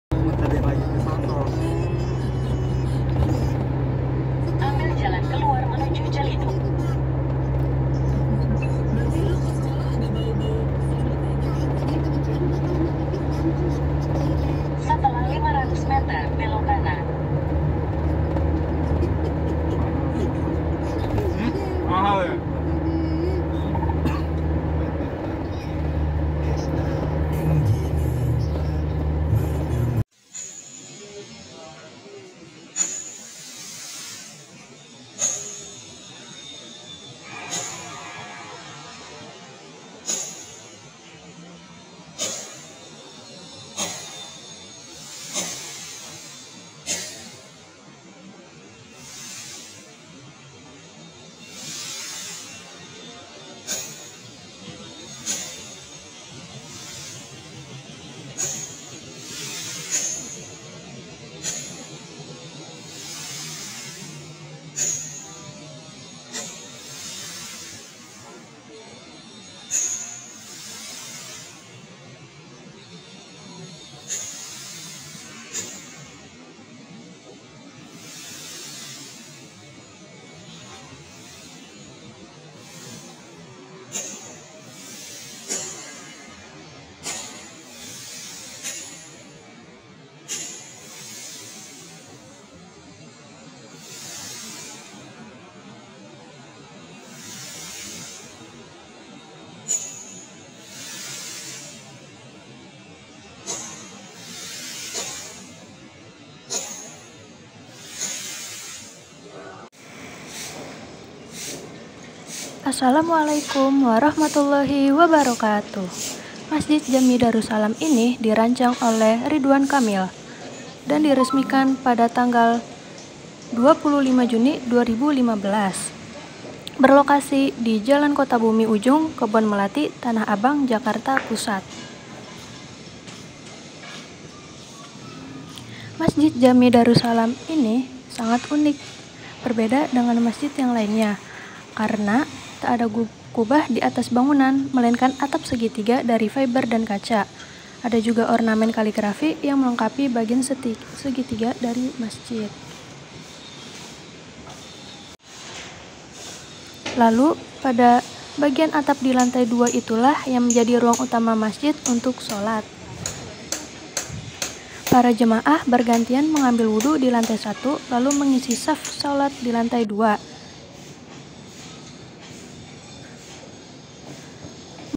Mengunjungi Masjid Unik Kubah Segitiga Jakarta
Namanya Masjid Darussalam,masjid unik kubah tiga yang berbeda dengan masjid yang umumnya, di mana kubahnya berbentuk bulat